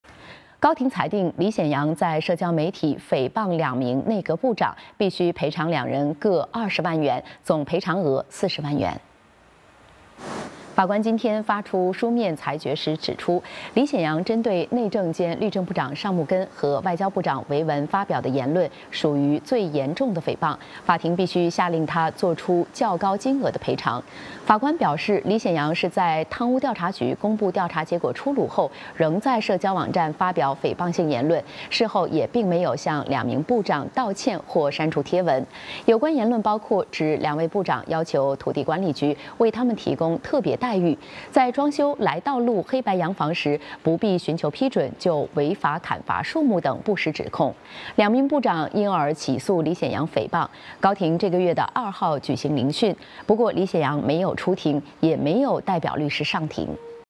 针对两名部长发表诽谤言论 高庭判李显扬赔偿总额40万元
高庭裁决李显扬在社交媒体诽谤两名内阁部长，须赔偿两人各20万元，总赔偿额40万元。